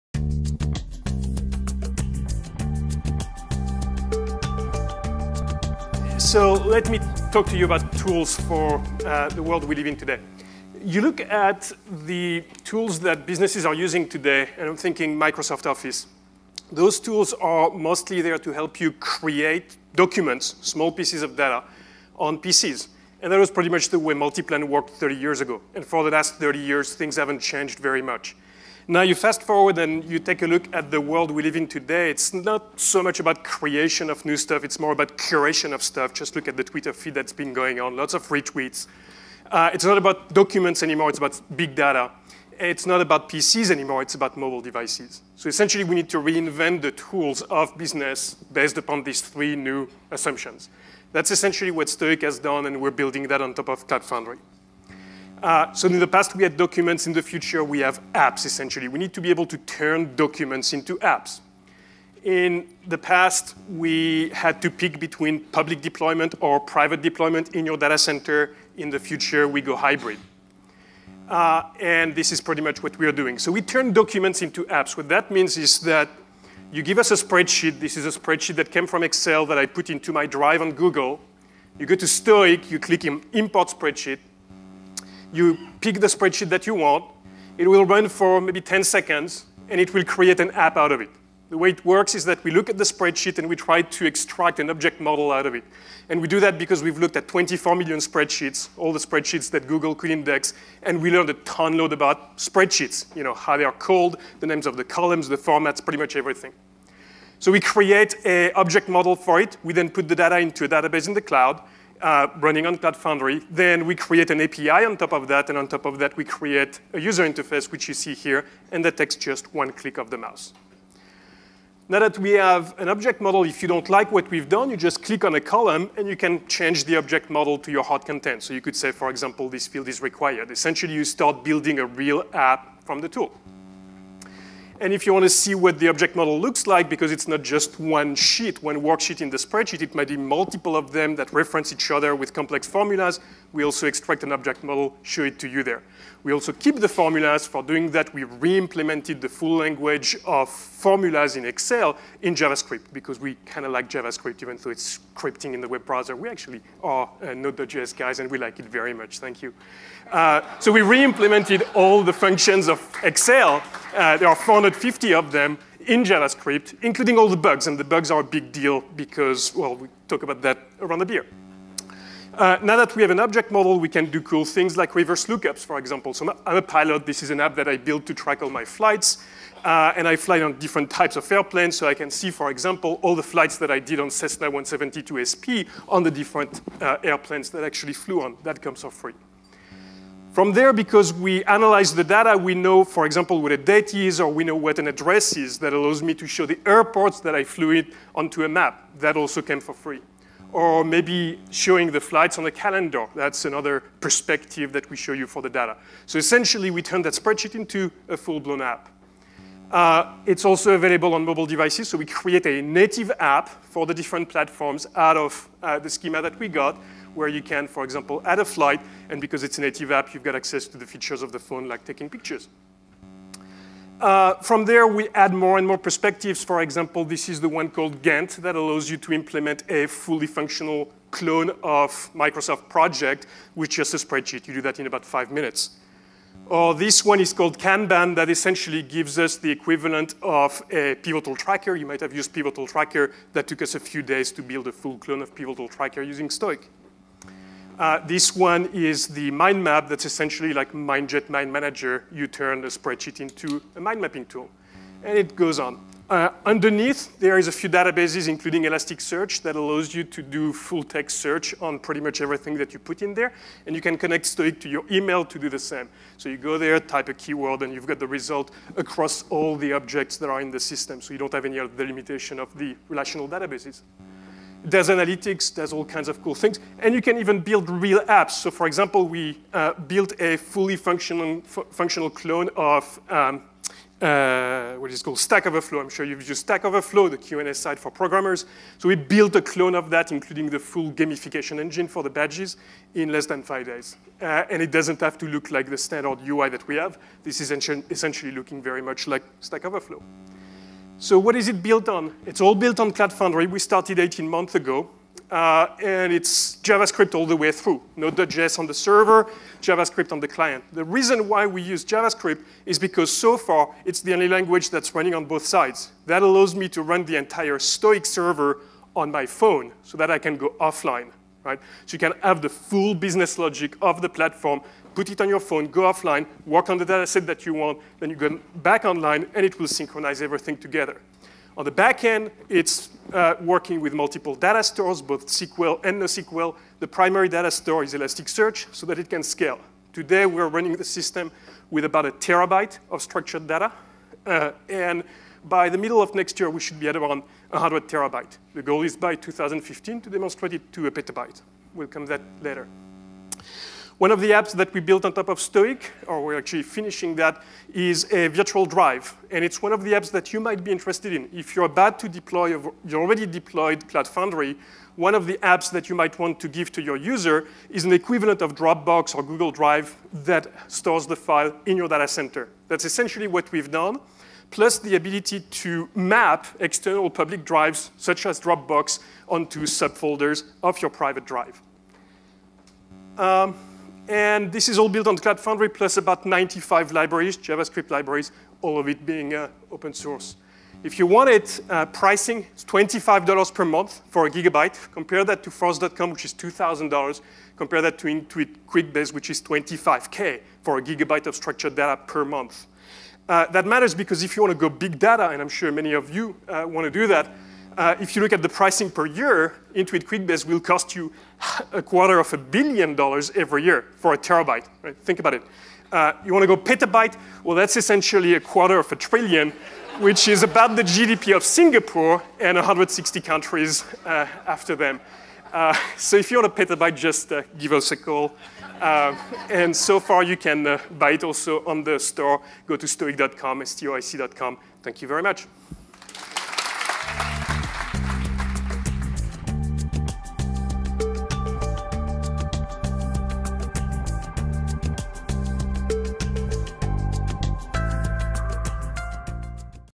New Tools for a New Era (Platform: The Cloud Foundry Conference 2013)
Keynote presented by Ismael Ghalimi, CEO, STOIC.

The traditional office productivity suite was built to let PC users create small documents for printing purposes. In the era of cloud computing, mobile users need a new set of tools that will help them curate big data for decision making and automation. STOIC is leveraging the Cloud Foundry platform to create these tools.